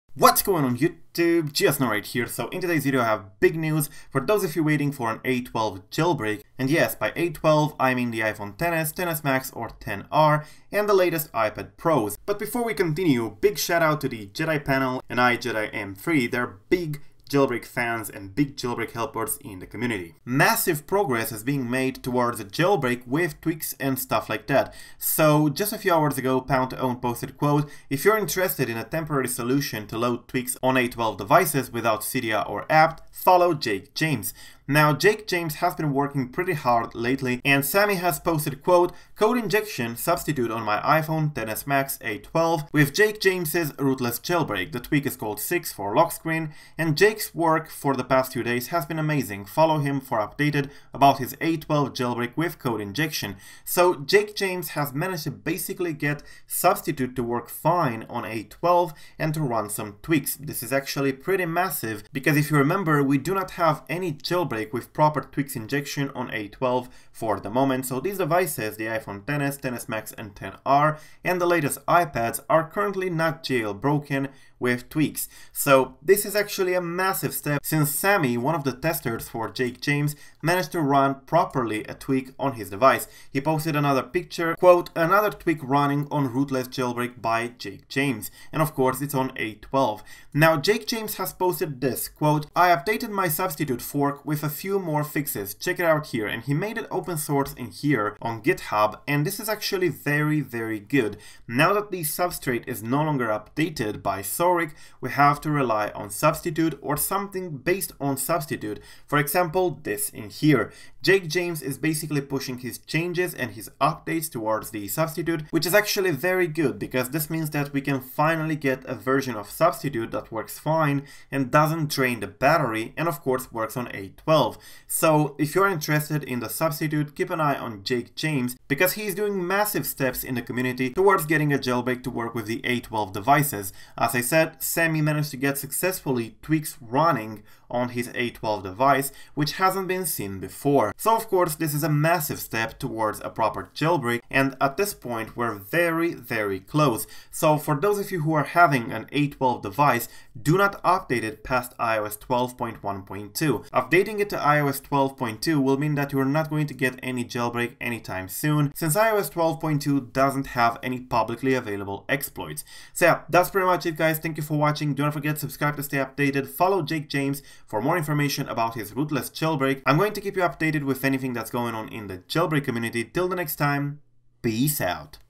RootlessJB Jailbreak (A12) Massive News | Tweaks Working! (iOS 12.0 - 12.1.2)
▶ iMyFone KeyGenius helps you unlock iTunes backup password and remove iPhone encryption settings.

In today's video, we're discussing the RootlessJB Jailbreak by Jake James. After impressing with its quick tweak support for iOS 12 back in February (at that point rootlessJb was the only jailbreak for iOS 12 supporting Tweaks), rootlessJB seems to be back with even more impressive achievements, this time for the A12 devices. The A12 devices (iPhone XS, XS MAX and iPhone XR) do not have a proper jailbreak with tweaks currently. It's been the longest unjailbroken iPhone series as far as I know. Well, things are about to change.

Sammy Guichelaar (@SammyGuichelaar), has posted a few hours ago a number of pictures with his A12 device running tweaks properly using RootlessJb. As far as I know, Sammy does testing for Rootless, so he might have received a testing build of Rootless by Jake James. It looks like Jake is closer than ever to having the first A12 Jailbreak with tweaks support. Pwn20wnd, one of the developers behind Unc0ver Jailbreak has posted a few hours ago on his Twitter account that people should, indeed, follow Jake if they're interested in a (temporary) solution for running tweaks on A12 devices.

Jake's method is implying his own fork of Substitute which is Open Source. This not only helps the current iOS 12.0 to 12.1.2 jailbreak but also the future iOS 12.1.3, iOS 12.1.4 and iOS 12.2 jailbreak since at that moment we'll hopefully have a properly updated Substitute to work with.